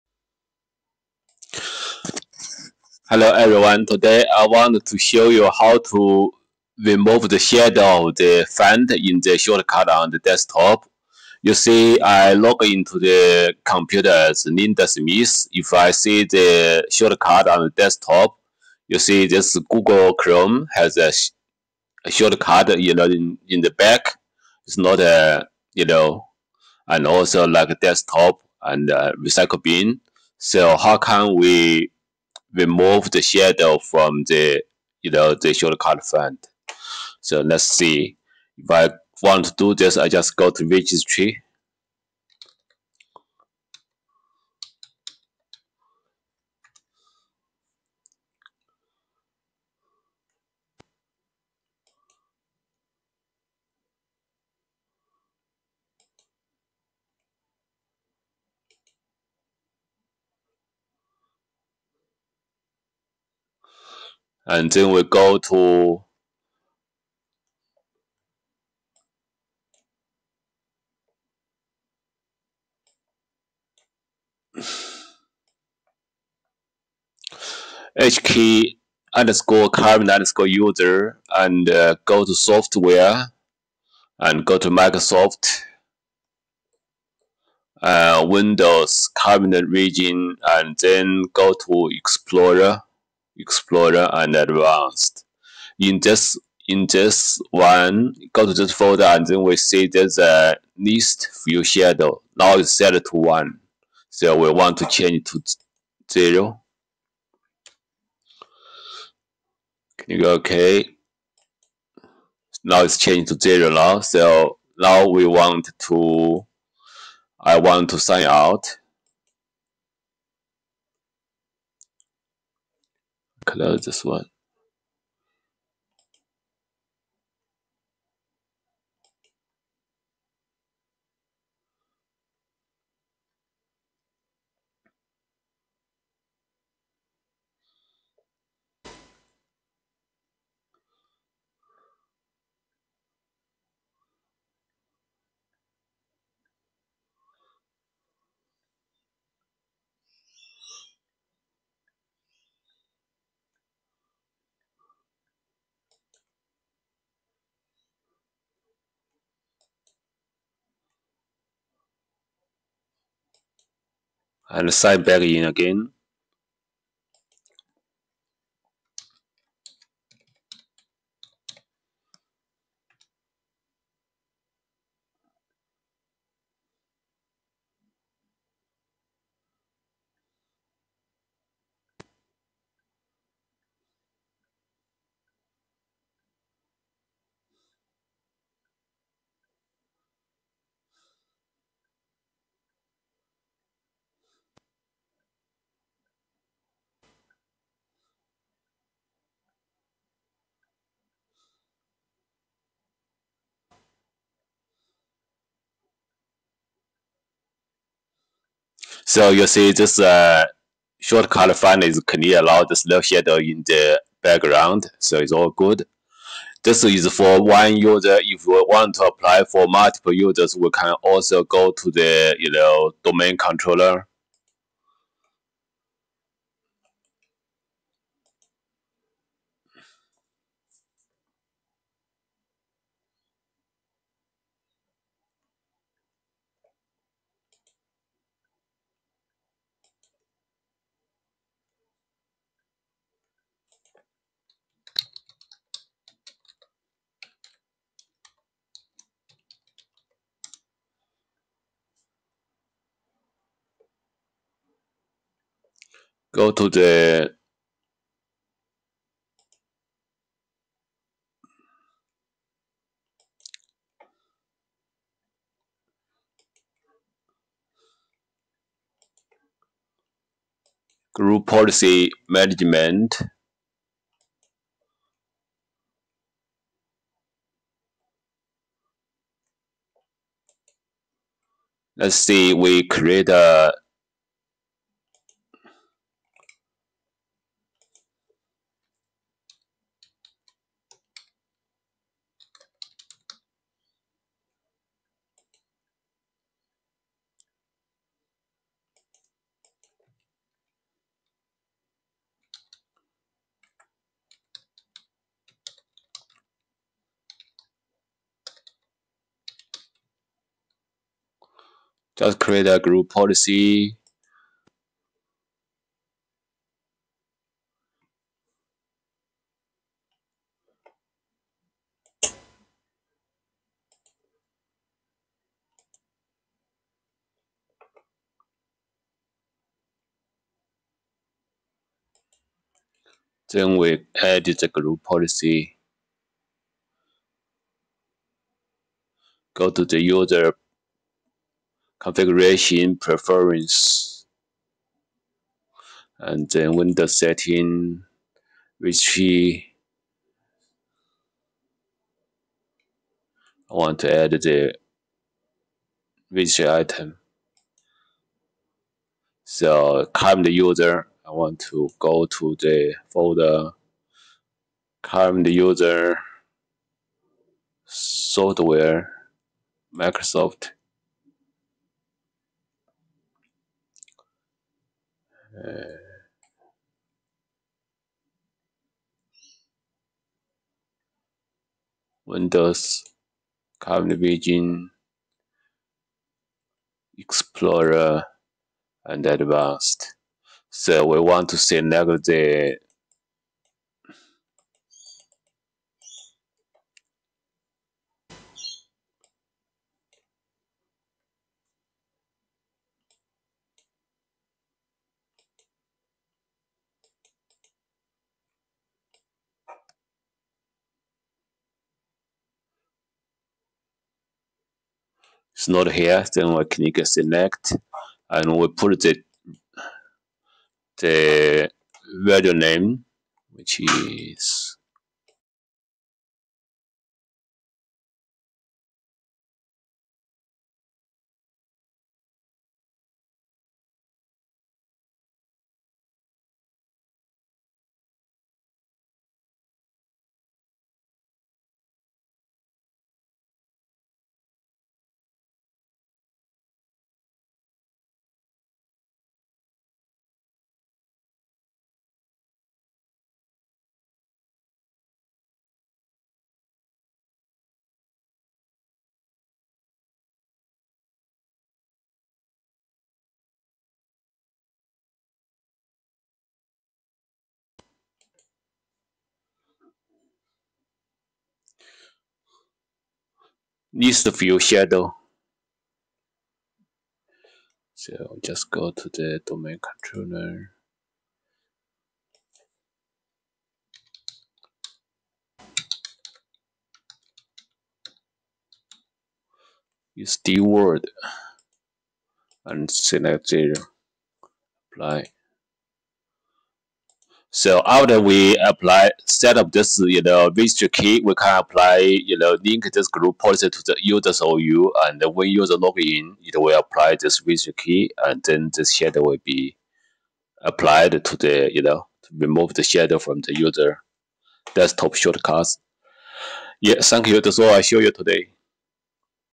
WIndows - How to remove shadow for the shortcuts on desktop
This video will show you how to remove shadow from shortcuts on user desktop